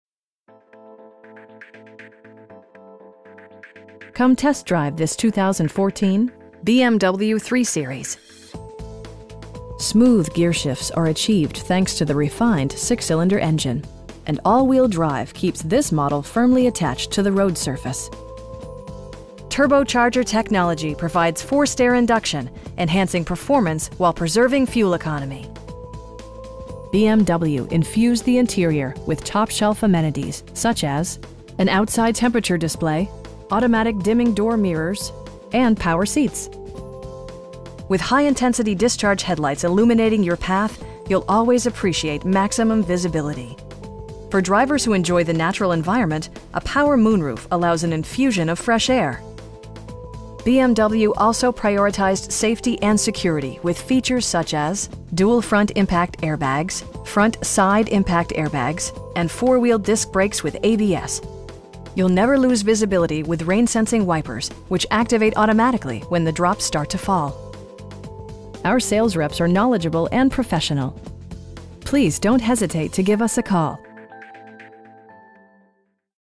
2014 BMW 335i xDrive in Dedham, MA 02026
Come test drive this 2014 BMW 3-Series. This 4 door, 5 passenger sedan still has less than 45,000 miles! Smooth gearshifts are achieved thanks to the refined 6 cylinder engine, and all wheel drive keeps this model firmly attached to the road surface. Turbocharger technology provides forced air induction, enhancing performance while preserving fuel economy. BMW infused the interior with top shelf amenities, such as: a built-in garage door transmitter, an outside temperature display, automatic dimming door mirrors, front bucket seats, tilt and telescoping steering wheel, cruise control, and power seats. With high intensity discharge headlights illuminating your path, you'll always appreciate maximum visibility. For drivers who enjoy the natural environment, a power moon roof allows an infusion of fresh air. BMW also prioritized safety and security with features such as: dual front impact airbags, front side impact airbags, traction control, brake assist, anti-whiplash front head restraints, a panic alarm, an emergency communication system, and 4 wheel disc brakes with ABS. You'll never lose visibility with rain sensing wipers, which activate automatically when the drops start to fall. Our sales reps are knowledgeable and professional. We'd be happy to answer any questions that you may have. Please don't hesitate to give us a call.